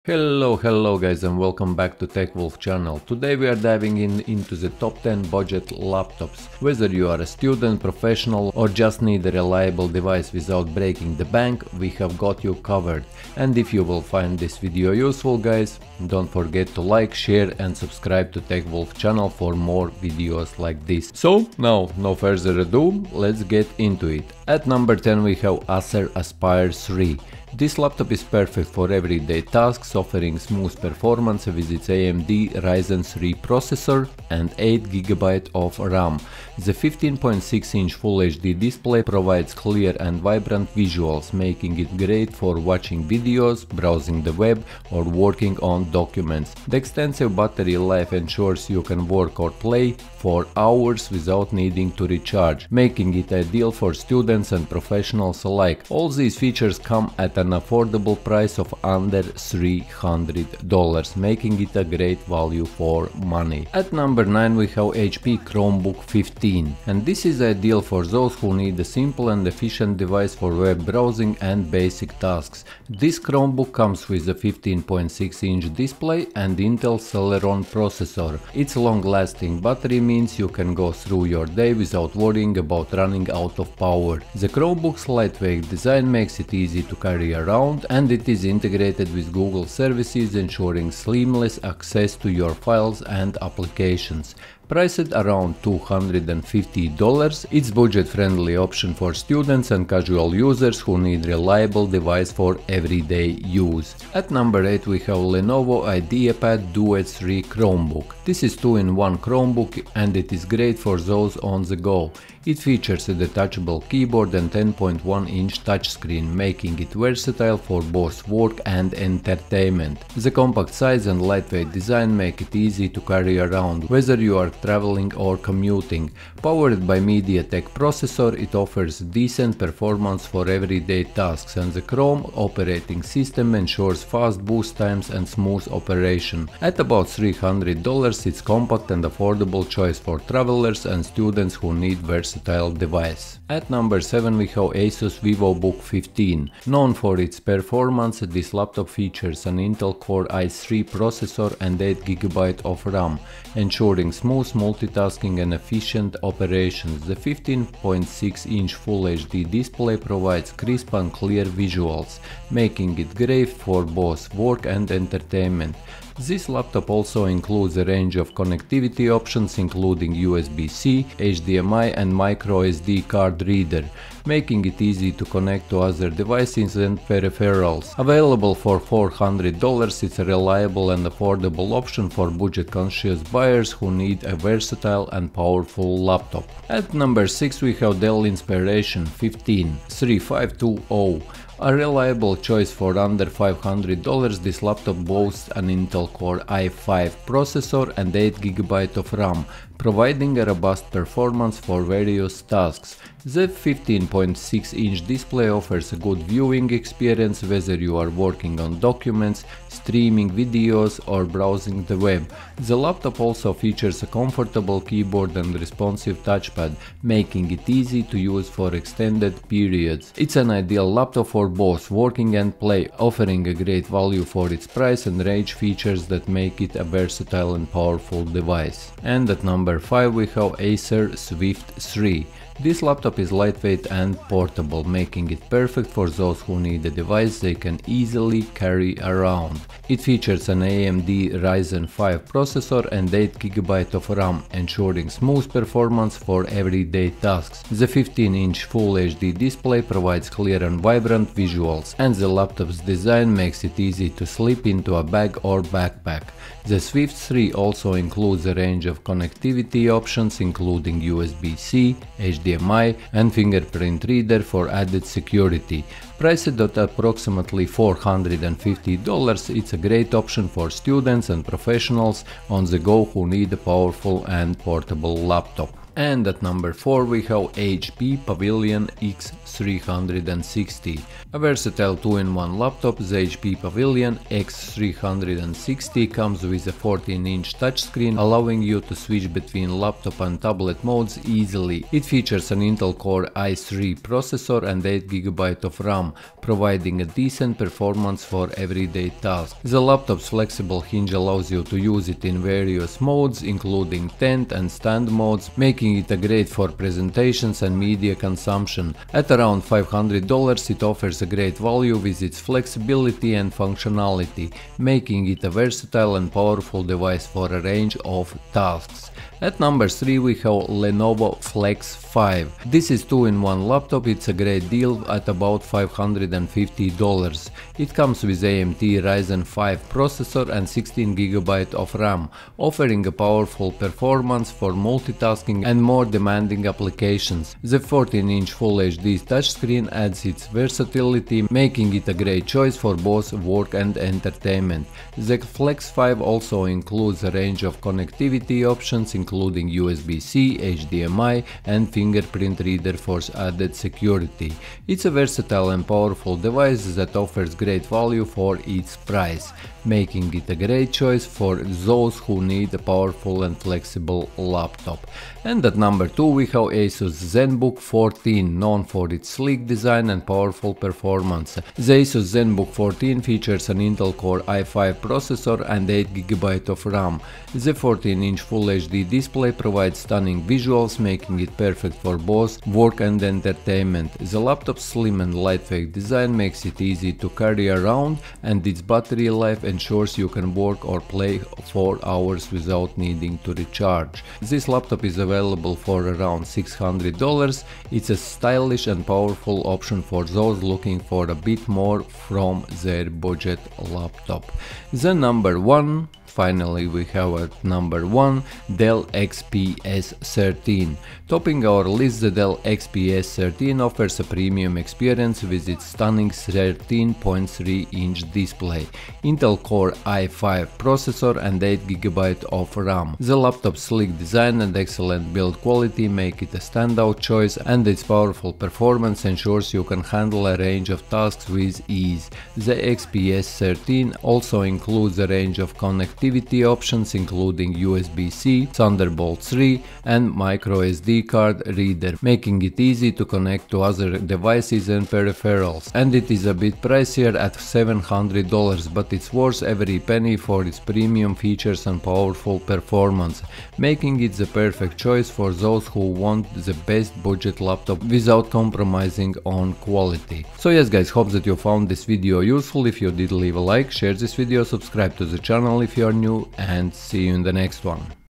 Top 10 Budget Laptops | Cheap Laptops
Are you looking for a budget-friendly laptop that offers great performance without breaking the bank? Look no further! In this video, we’ll explore the top 10 budget laptops of 2024, perfect for students, professionals, and anyone in need of a reliable device. Let’s dive in!

Time Stamp:
00:00 Introduction
00:25 Acer Aspire 3 Laptop
01:04 HP Chromebook 15
01:44  Lenovo IdeaPad Duet 3 Chromebook
02:24 ASUS Vivobook 15
03:07 Dell Inspiron 15 3520
03:50 Acer Swift 3
04:37 HP Pavilion x360
05:20 Lenovo Flex 5
06:06 ASUS ZenBook 14
06:48 Dell XPS 13
07:40 Outro